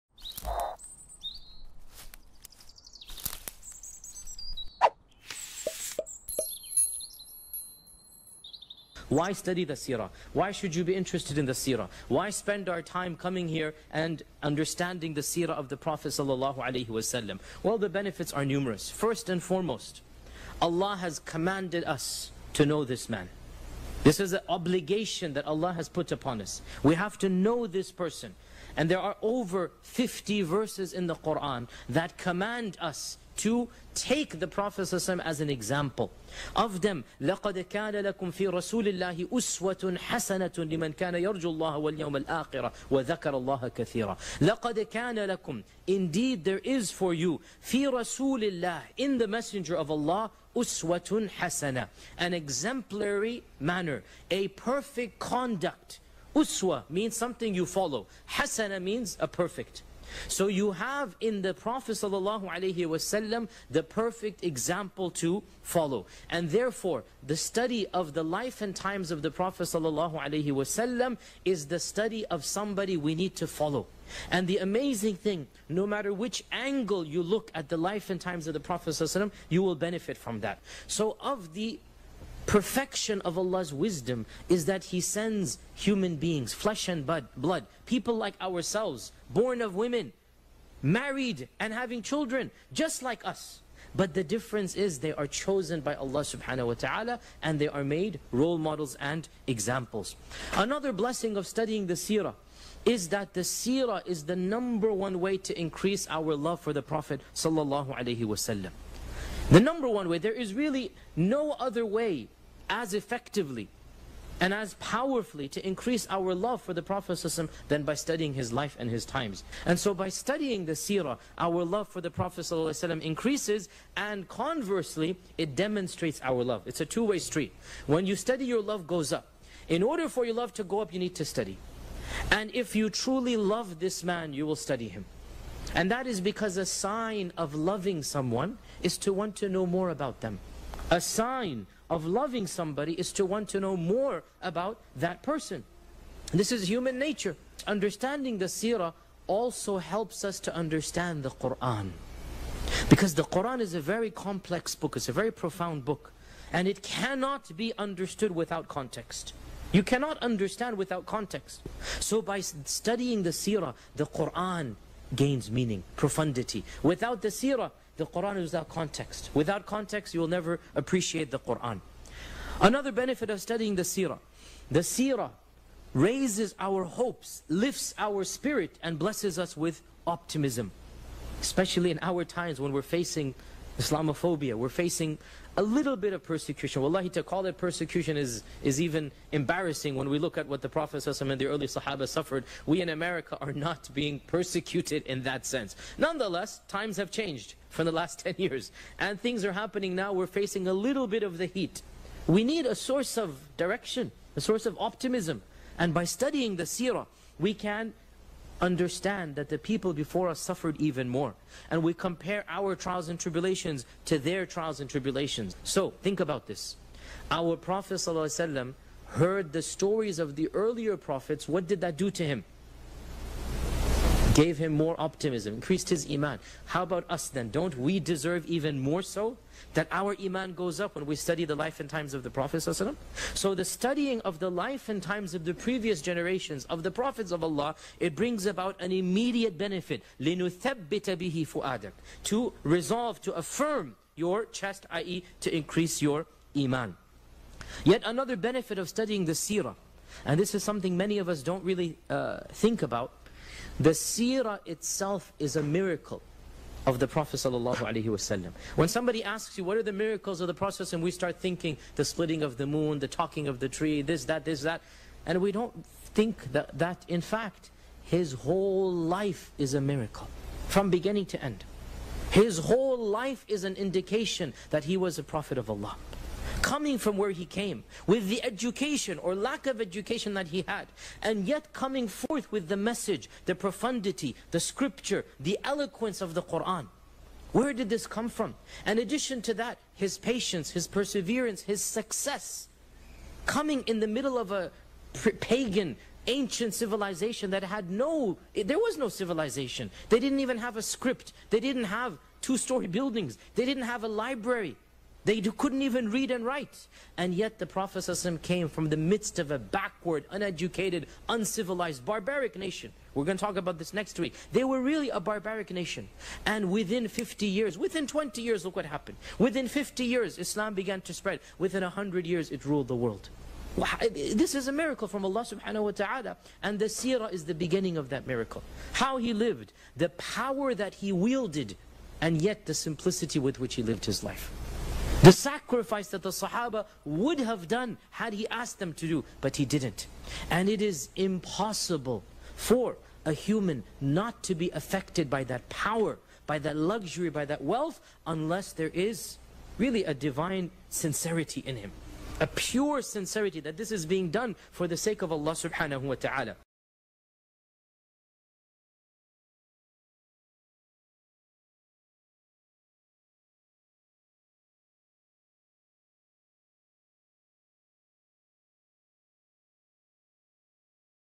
Ep 3: Why Study Seerah? | Lessons from the Seerah | Shaykh Yasir Qadhi
Why study the Seerah? Why should you be interested in the Seerah? Why spend our time coming here and understanding the Seerah of the Prophet (ﷺ)?

Share and Help us spread the Message of Lessons from the Seerah series. - @YasirQadhi

Watch the Complete Lessons from the Seerah Series by Shaykh Yasir Qadhi

CHAPTERS
0:00 Why Study Seerah?
0:20 Allah has commanded us to know this man
1:40 Another blessing of studying the Seerah
2:43 The Quran gains meaning, profundity
4:13 The Seerah itself is a miracle of the Prophet (ﷺ)
5:56 Donation